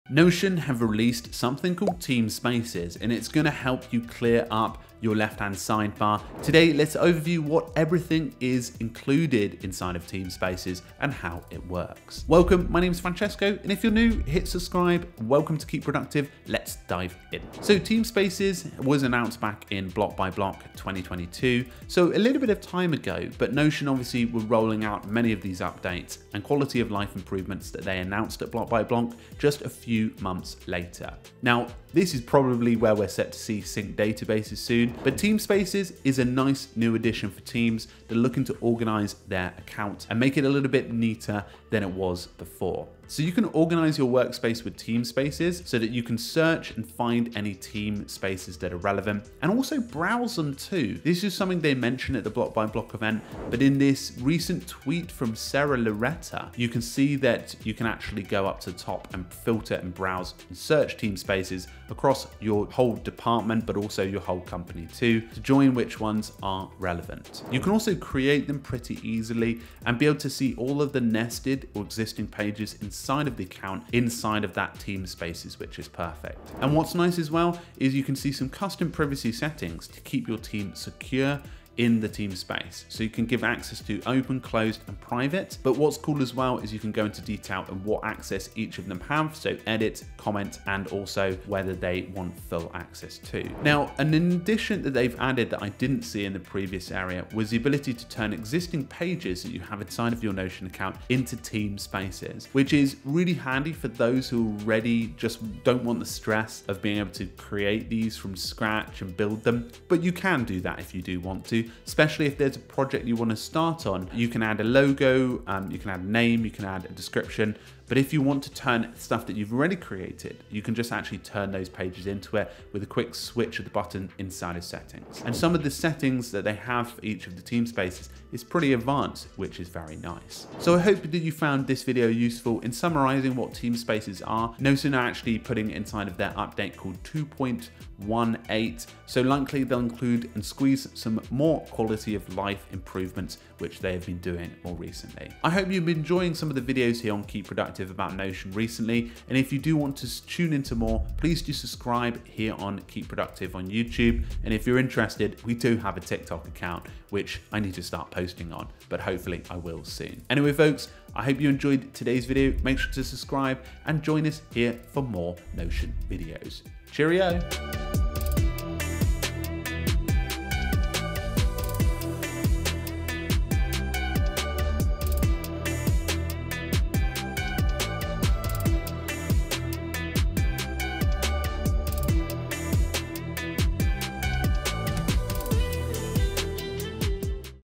Notion Teamspaces: Everything You Need to Know
Stop doing busywork! Try Bento Focus Find the best productivity tools with our Notion have introduced better team collaboration with Notion teamspaces - for team accounts. Let's explore what they are, how Teamspaces work and what they do...

00:00 - Introduction
00:32 - What are Notion Teamspaces?
01:01 - Additional Features
02:30 - Summary

💻  FINAL THOUGHTS